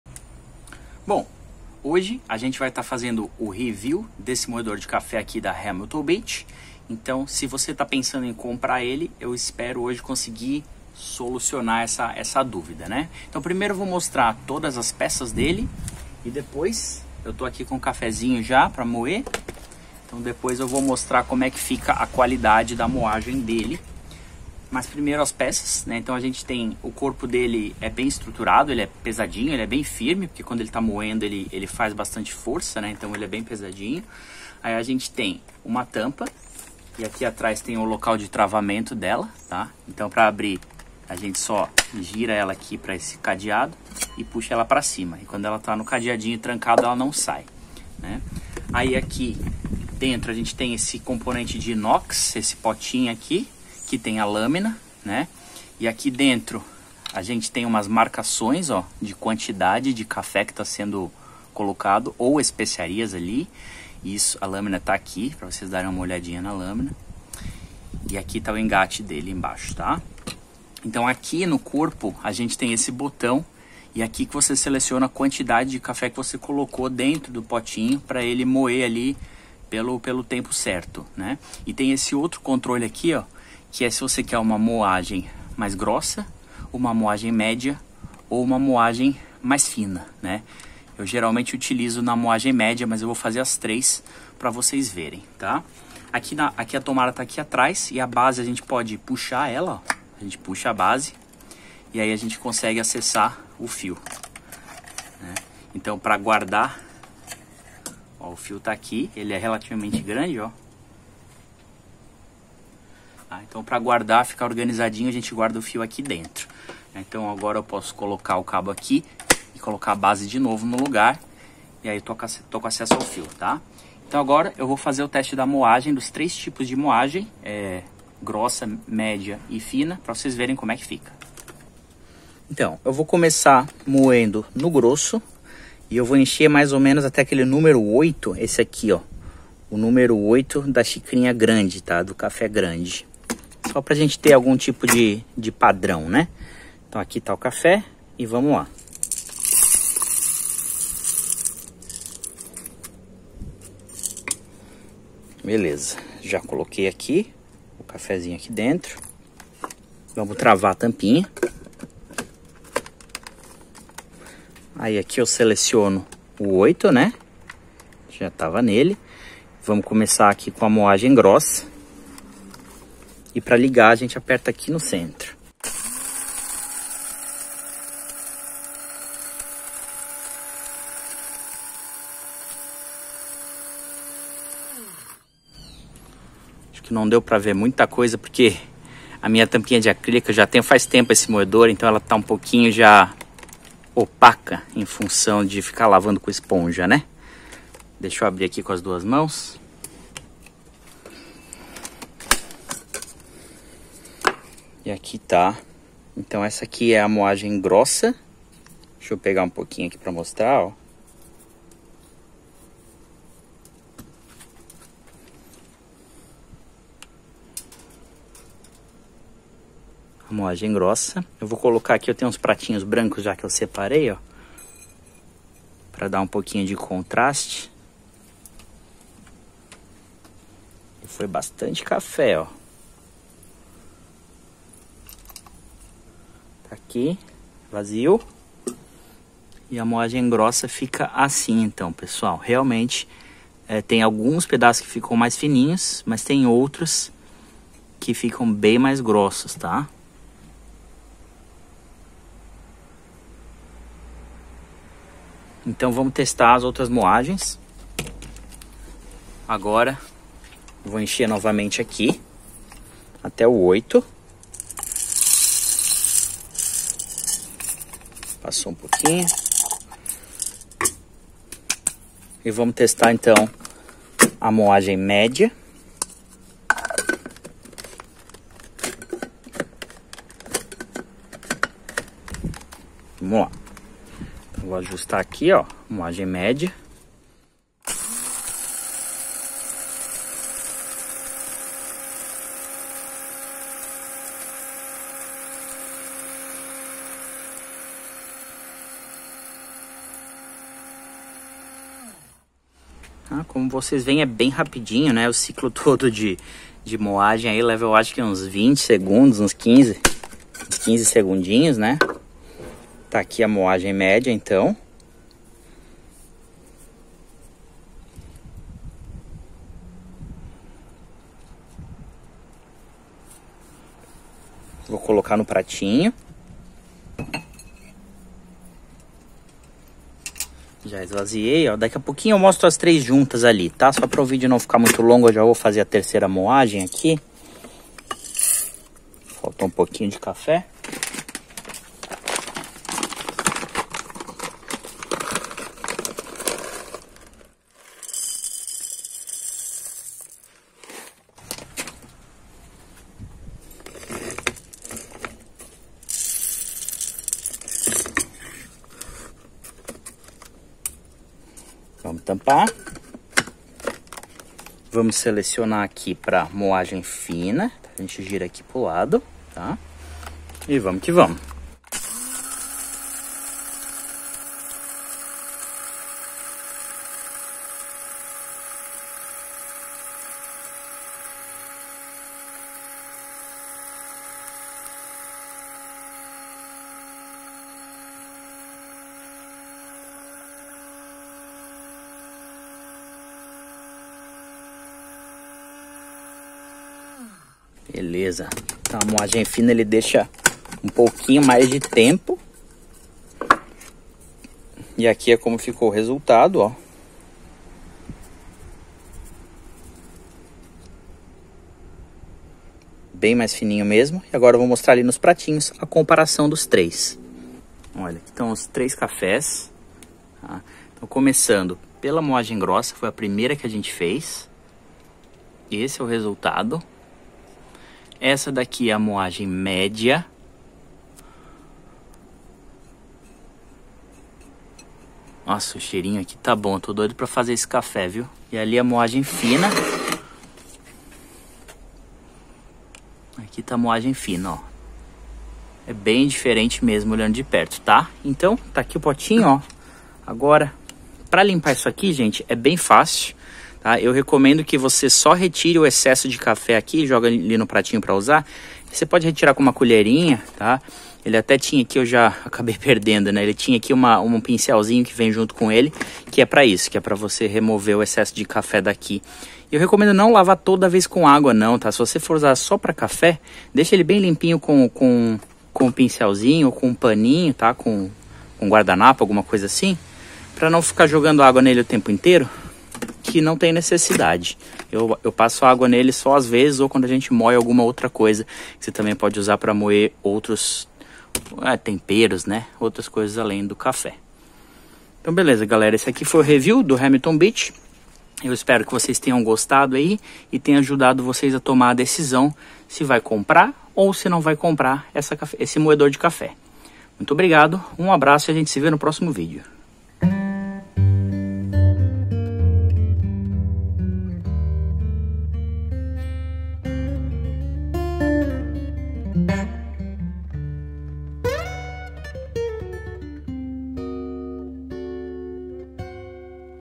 Moedor de Café Hamilton Beach (Review, análise) | Teste de Moagem
Neste vídeo, o André está mostrando o moedor de café da marca Hamilton Beach. Ele inicia o vídeo mostrando as peças que compõe o moedor, depois mostra as funcionalidades (botão de quantidade de café e botão espessura da moagem) e então realiza 3 testes de moagem para verificar as diferentes configurações de moagem do moedor Hamilton Beach.

VEJA O PRODUTO NA AMAZON
--- Este é um modelo um pouco mais caro, porém superior em qualidade!

00:00 Apresentação do Moedor
02:11 Moagem Grossa
04:17 Moagem Média
05:30 Moagem Fina
06:52 Comparação das 3 moagens

VEJA OUTROS VÍDEOS LEGAIS DO NOSSO CANAL